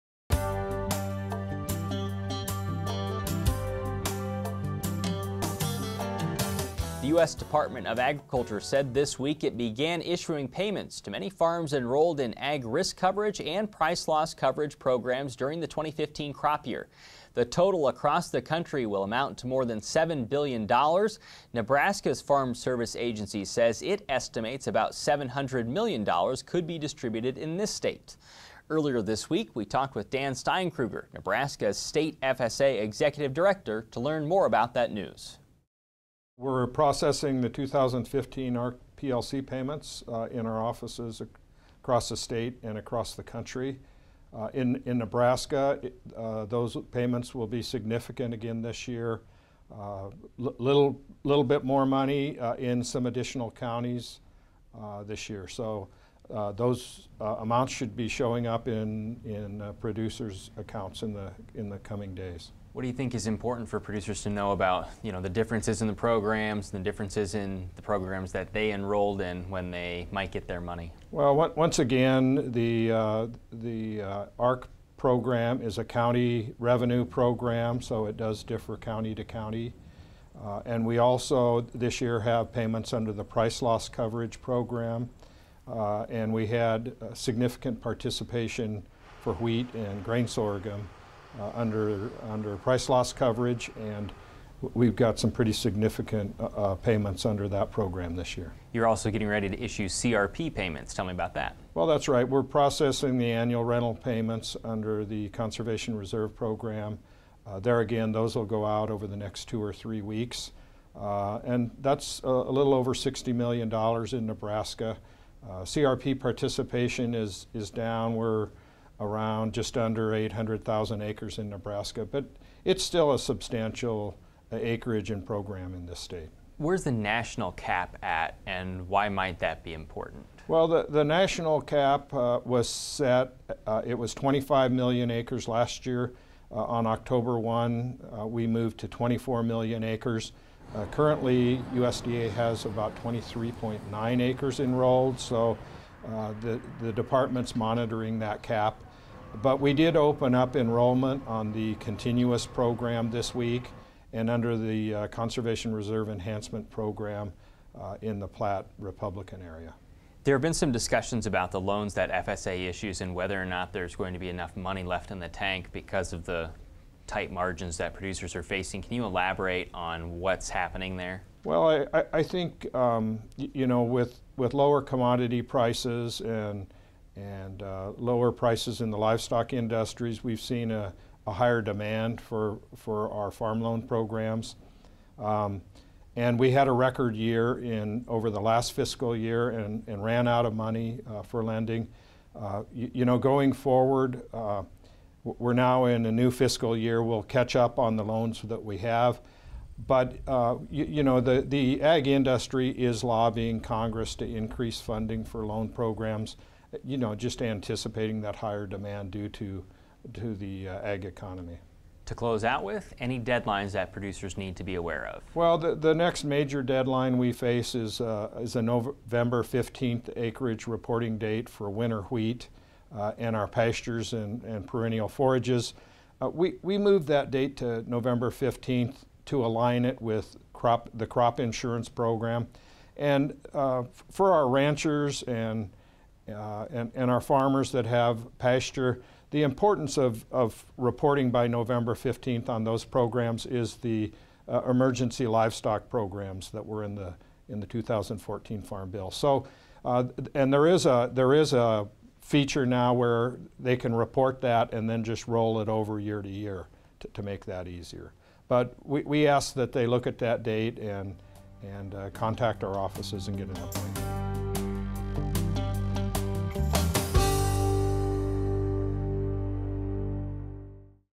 Farm Program Payments - Dan Steinkruger - October 7, 2016
Dan Steinkruger, Nebraska State Farm Service Agency executive director, talks about farm program payments under Agriculture Risk Coverage and Price Loss Coverage. Dan also discusses approaching deadlines for producers across the state.